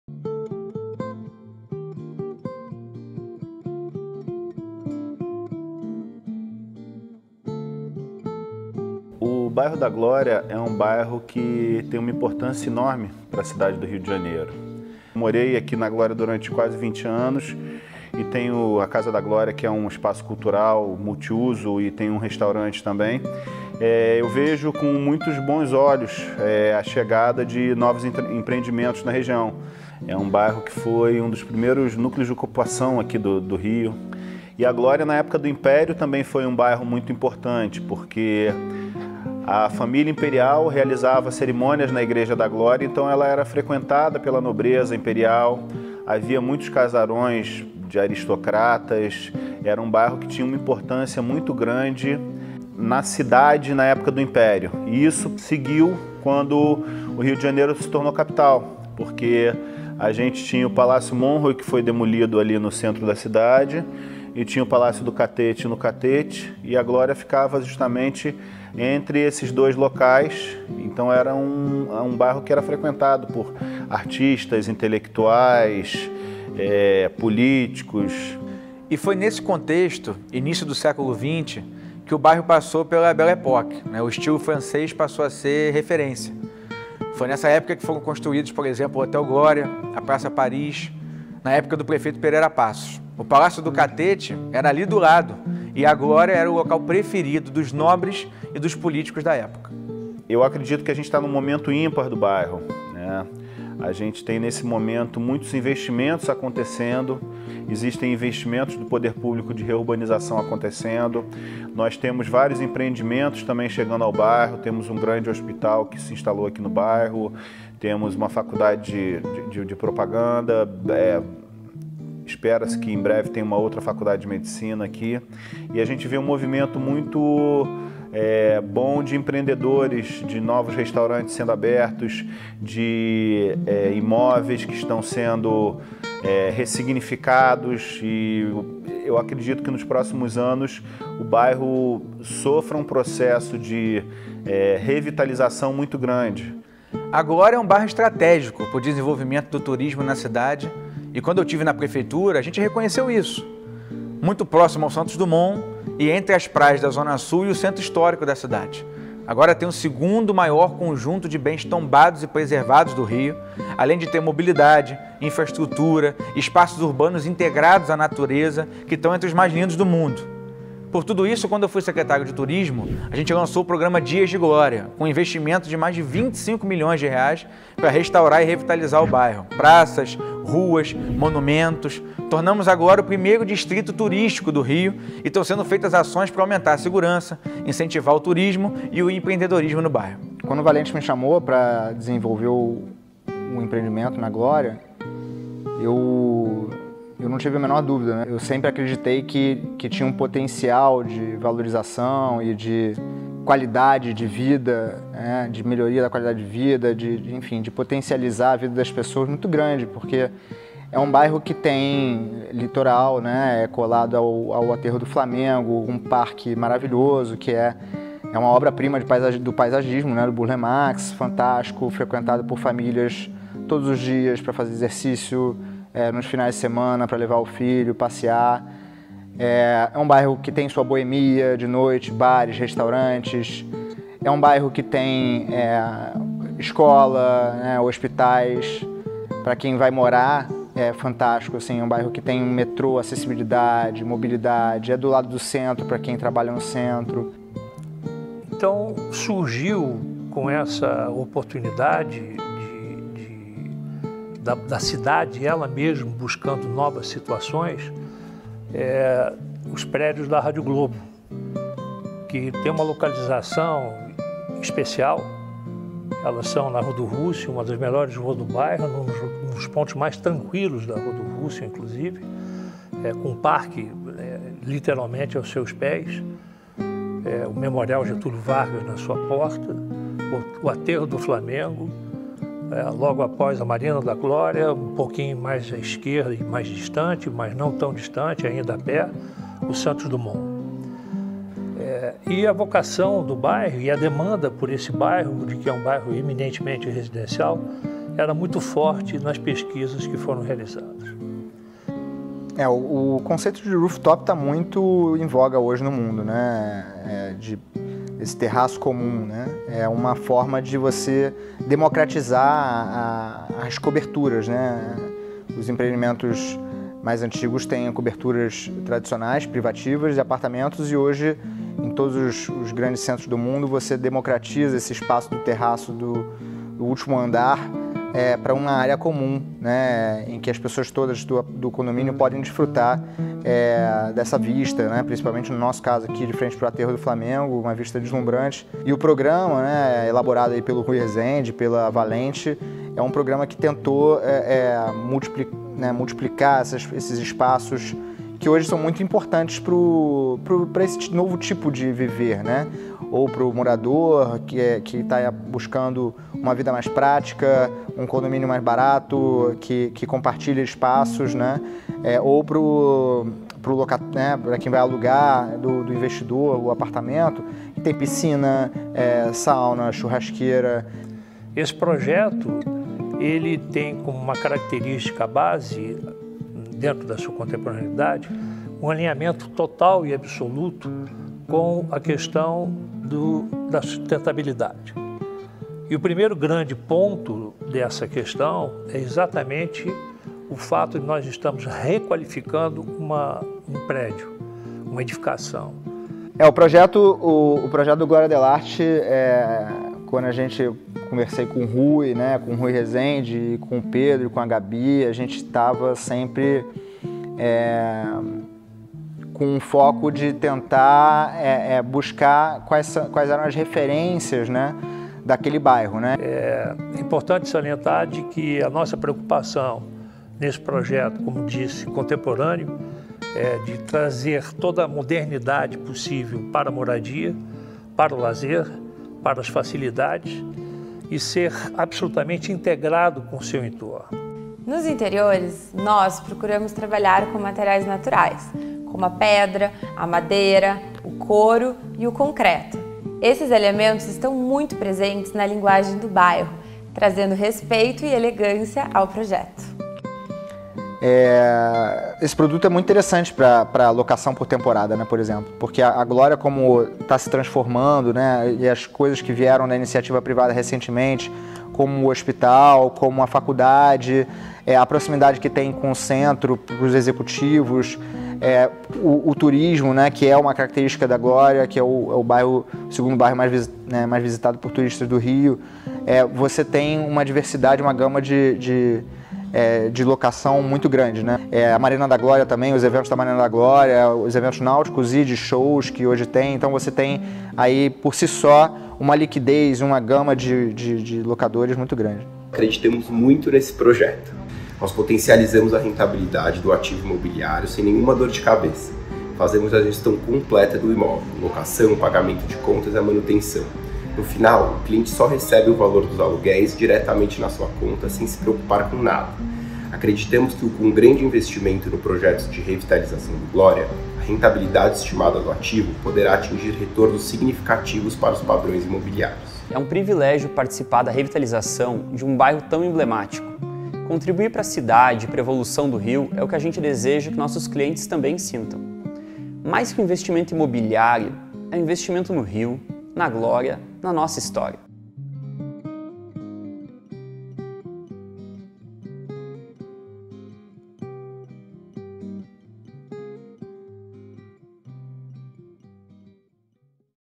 CONHEÇA GLÓRIA DEL ART CO LIVING
Lançamento Residencial: Glória Del Art Co Living

O Glória Del Art Co Living é um lançamento residencial situado no bairro Glória, em uma das regiões mais valorizadas da Zona Sul carioca. O empreendimento é composto por imóveis acolhedores e inúmeros diferenciais em serviços para tornar a vida dos moradores muito mais prática, e apresenta um excelente custo-benefício para a aquisição dos apartamentos.
O condomínio fica próximo ao antigo Hotel Glória e ao Aterro do Flamengo, em uma região repleta de opções de comércios, prestadores de serviços e transporte público. As ruas e avenidas importantes do entorno contribuem com a mobilidade urbana de todos, pois oferecem alternativas para melhorar o fluxo de trânsito nos deslocamentos entre bairros.

Não perca esta oportunidade de morar na Glória!

0:00 - Apresentação;
5:12 - Rooftop;
5:18 - Espaço Gourmet;
5:20 - Lavanderia;
5:23 - Coworking;
6:11 - Vista Rooftop;
6:35 - Fachada;
7:20 - Studios;
7:29 - Sala e quarto;
8:44 - Hall;
8:55 - Encerramento.